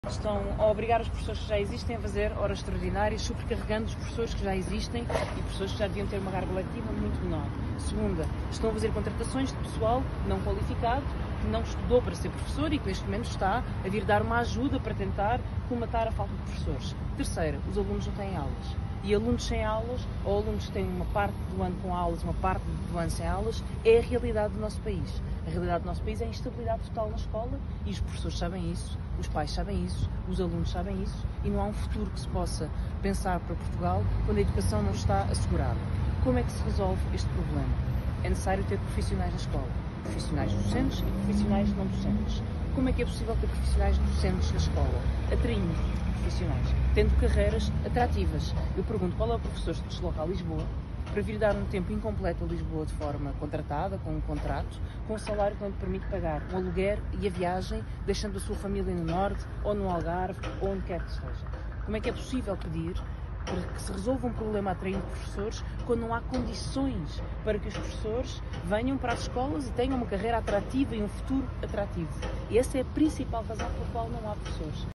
O salário de um professor deslocado não paga uma renda em várias cidades do país.
Com a ajuda do PSD, o PS impediu a recuperação do tempo de serviço dos professores. Ao mesmo tempo, o salário de um professor deslocado não paga uma renda em várias cidades do país. O resultado está à vista: milhares de alunos sem aulas. Aqui estão soluções urgentes para este problema.